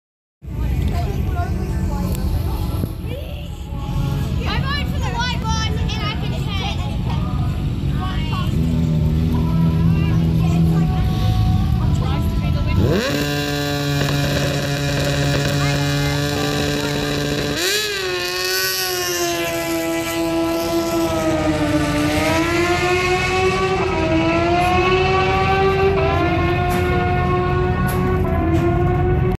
ZX10R Blows it's clutch
You know that moment you blow your clutch and it starts to sound like a CVT? This is it. Sorry about the potato camera.

Video Recording: GoPro Hero 3 Black Edition
Audio Recording: Sena BACPAC (GP10)
Editing Program: Lightworks
Intercom: Sena 20S

Scott: 2014 Kawasaki Ninja ZX-10R (ABS)